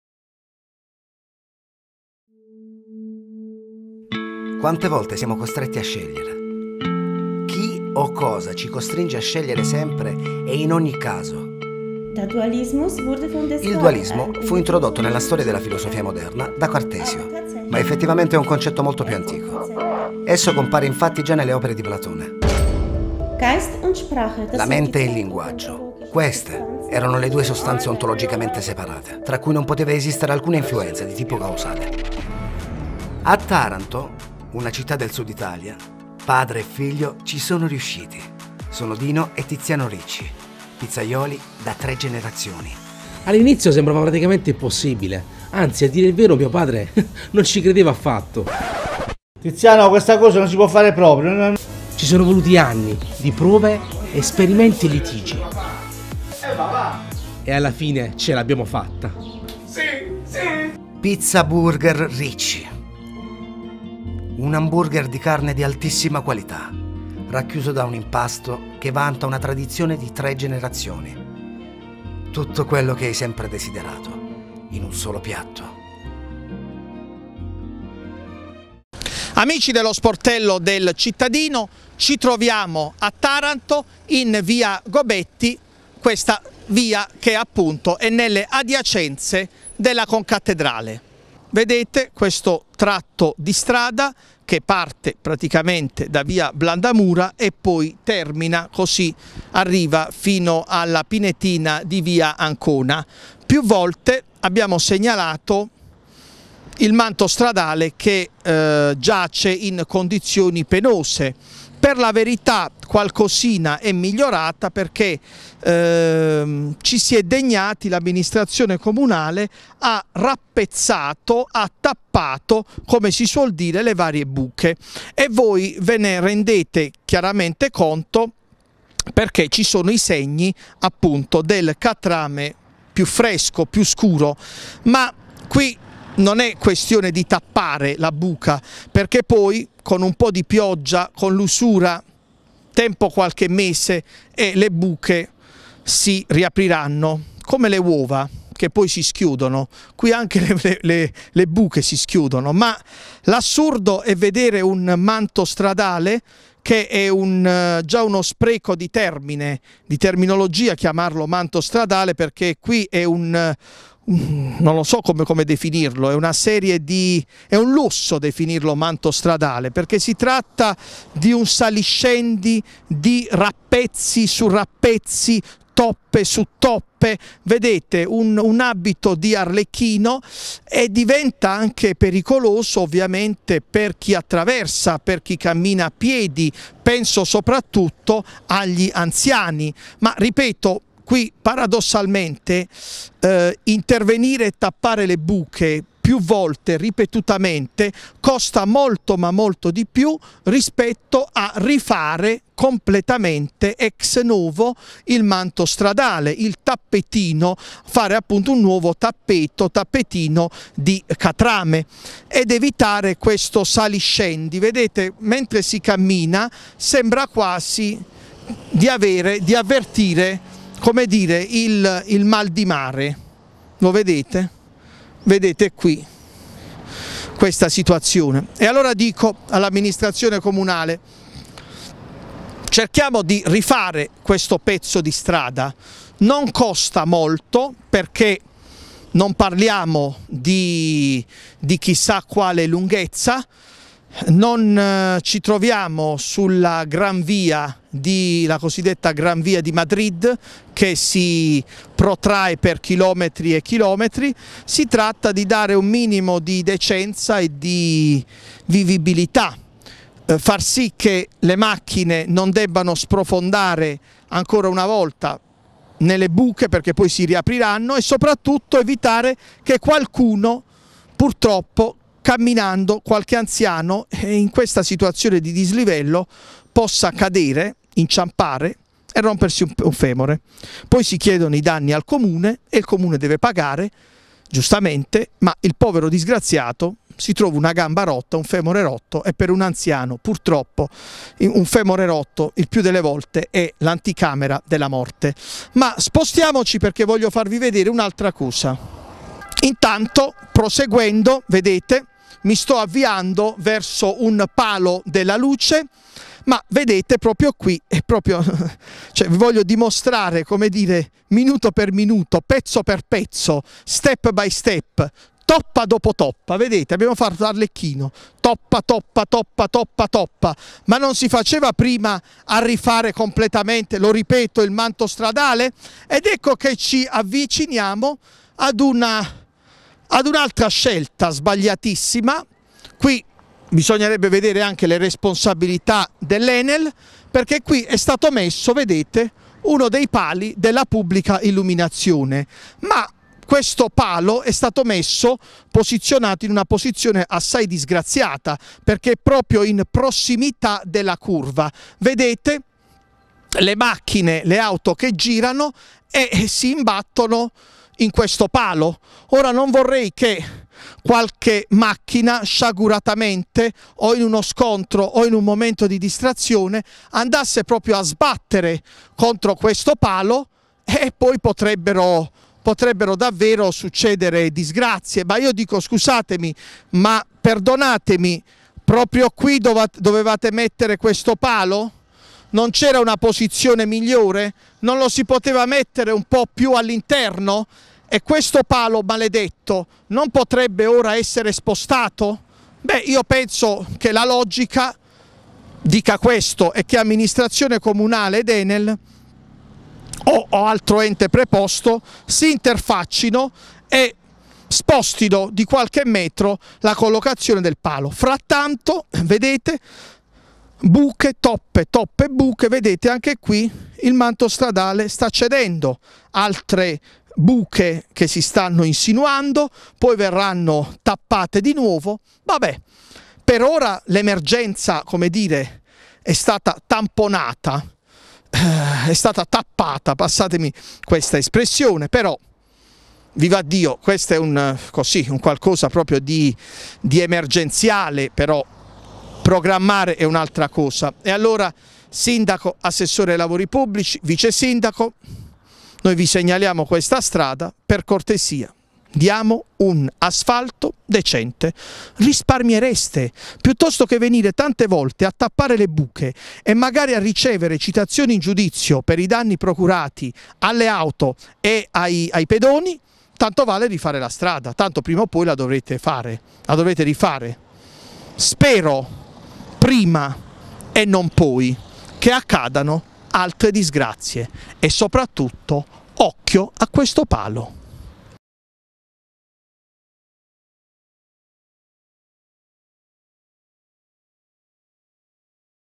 TARANTO - VIA GOBETTI (VICINANZE CONCATTEDRALE) - MANTO STRADALE DISSESTATO E RAPPEZZATO
TARANTO - VIA GOBETTI (VICINANZE CONCATTEDRALE) - MANTO STRADALE DISSESTATO E RAPPEZZATO - UN PALO PERICOLOSO IN CURVA